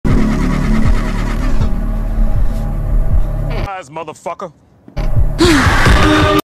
They Are Here Alien Abduction Horror surprise
a big surprise for a nosey Journalist